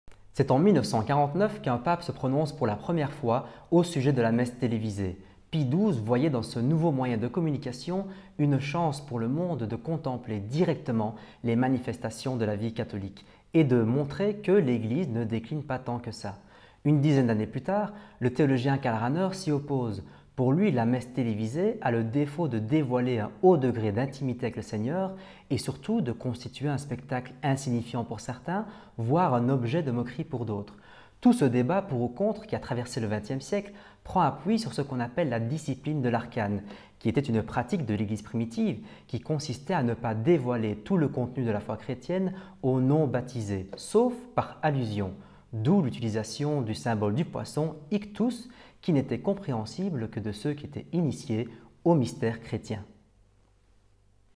La discipline de l’Arcane
La discipline de l'Arcane à partir du Petit dictionnaire de théologie catholique de Karl Rahner

Premier short de la chaîne !

N'hésitez pas à laisser des commentaires sur le contenu ou la forme.
Et surtout, dites-nous si le format vous plaît :-)